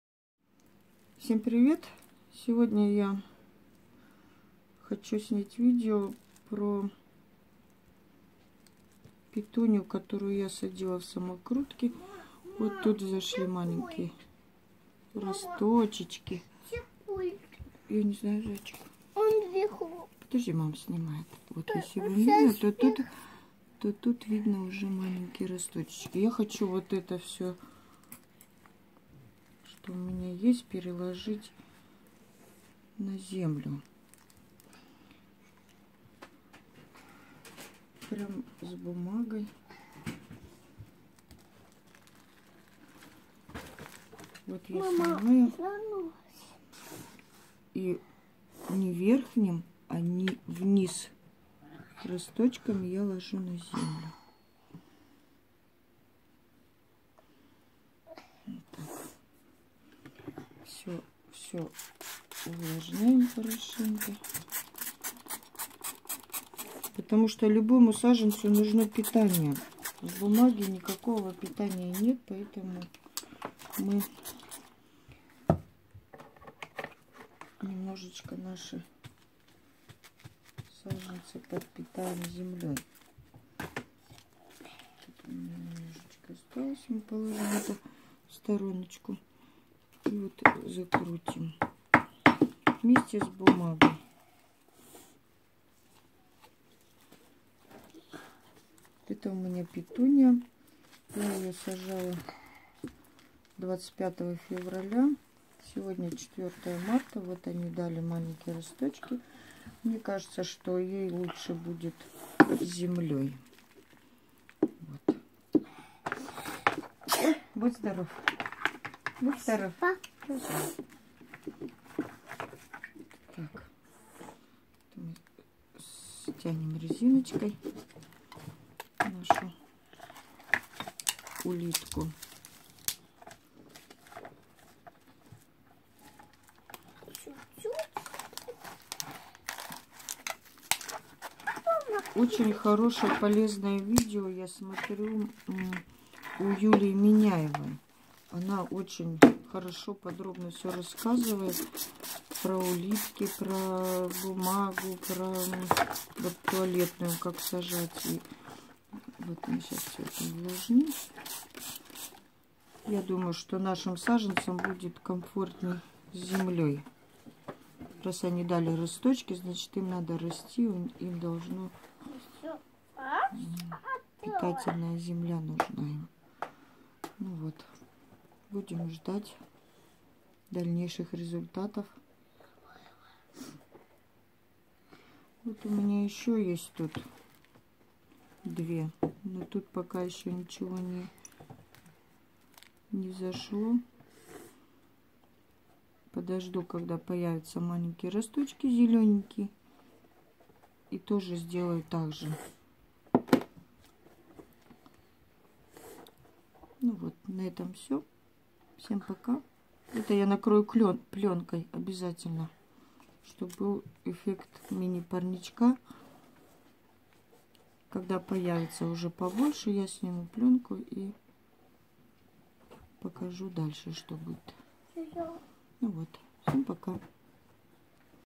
Петуния переезжает из самокрутки в улитку
Туалетная бумага не может дать нашим всходам питания,поэтому добавляем питательную землю любым способом,можно развернуть самокрутку и добавить грунт,либо попробовать вот таким методом,приятного цветения вам